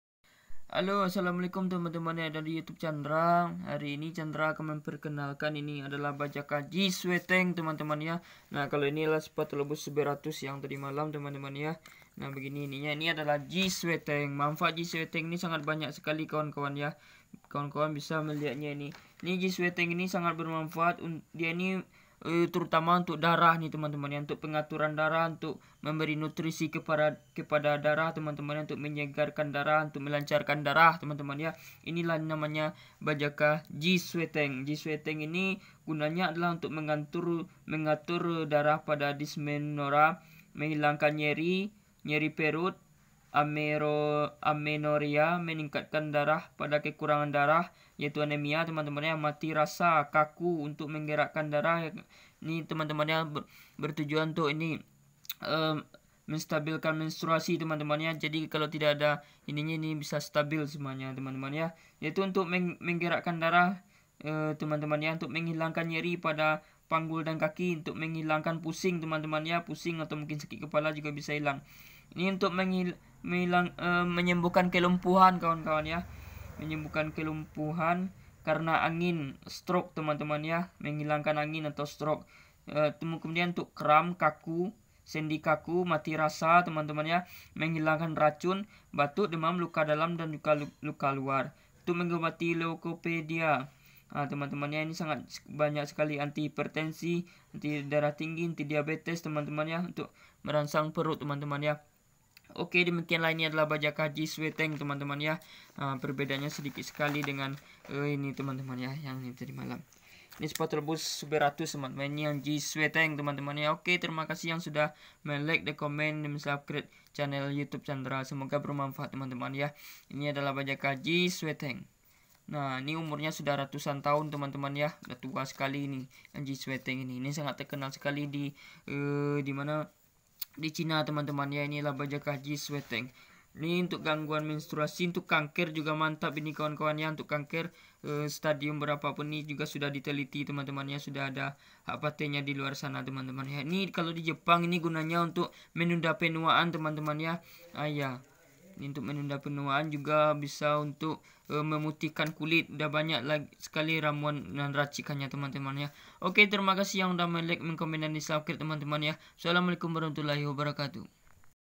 BAJAKAH JIXUATENG (CAULIS SPATHOLOBI).
Jixuateng bermamfaat
1.memberi nutrisi pada darah
2.menyegarkan darah
3.pembekuan darah
4.melancarkan darah
5.mentruasi tidak teratur
6.anti kanker
7.anti virus
8.anti tumor
9.anti biotik
10.anto oksidan
11.nyeri perut
12.amenorea
13.anemia
14.mati rasa
15.kaku kebas
16.anti nyeri pada panggul dan kaki
17.menghilangkan pusing
18.menghilangkan angin
19.strok ringan /berat
20.menghilangkan racun
21.batuk
22.demam.
23.luka luar luka dalam
24.leukopenia
25.obat penenang
26.diabetes
27.hipertensi
28.peradagan
29.anti penuaan
30.pemutih kulit
31.kelumpuhan
32.stamina
33.kekebalan tubuh
34.dll
Jixueteng adalah ramuan cina.pemesanan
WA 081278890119